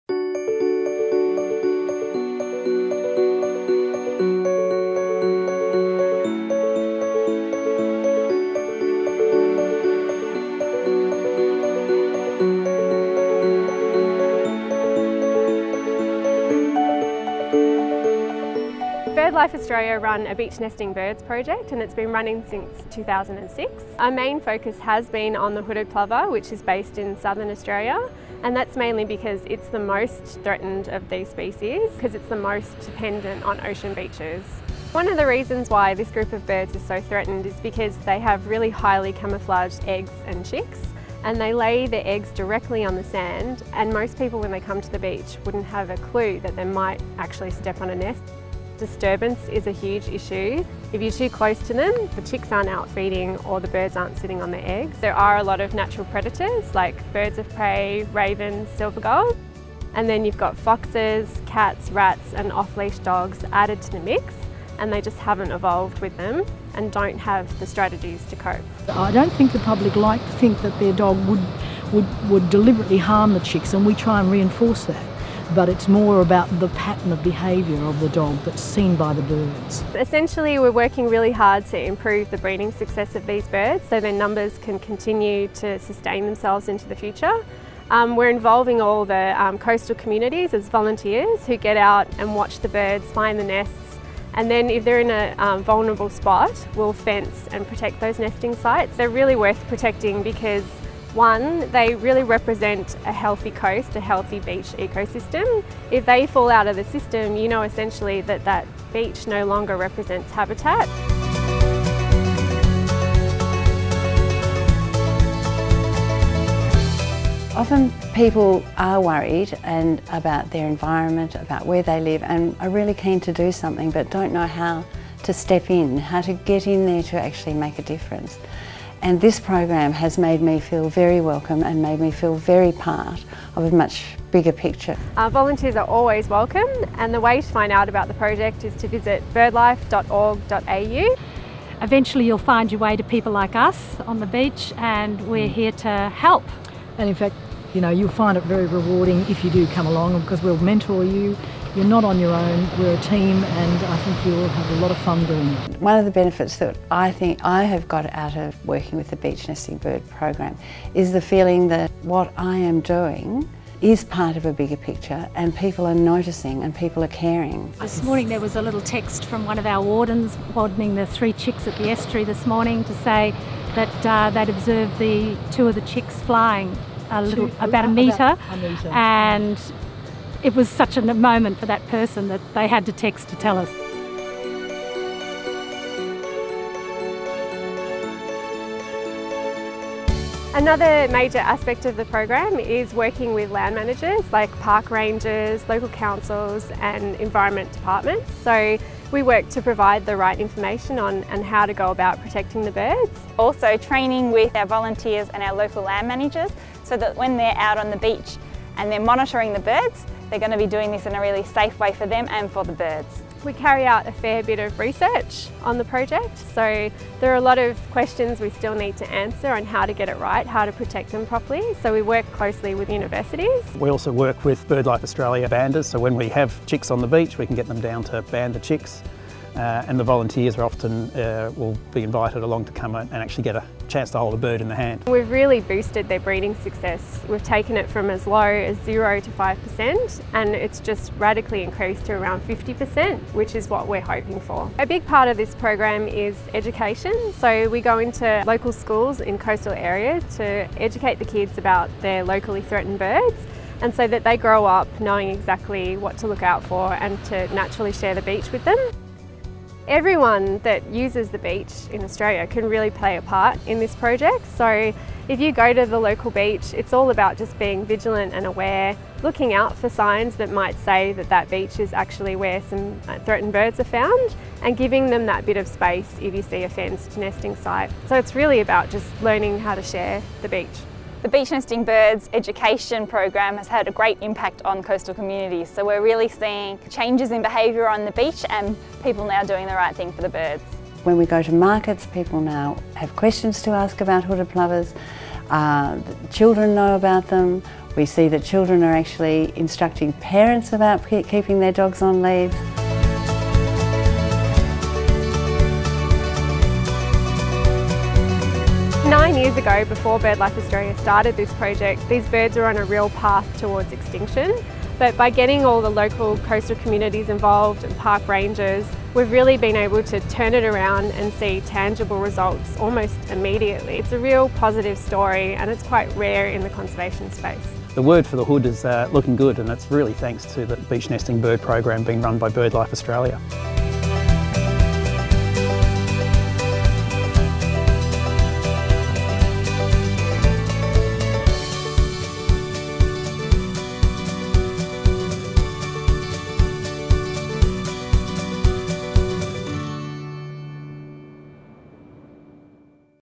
Beach-nesting Birds Project
This mini-documentary is about BirdLife Australia's Beach-nesting Birds project. This amazing group of birds depend on coastal habitats, which happen to be people's favourite place for recreation. These birds consequently are struggling to produce young in such disturbed environments. BirdLife Australia initiated a project of research, monitoring, on-ground works and education to bring them back from the brink of extinction. This is the story of building coexistence between birds and people on beaches. This is just the beginning of the journey, but already it's a tremendous success!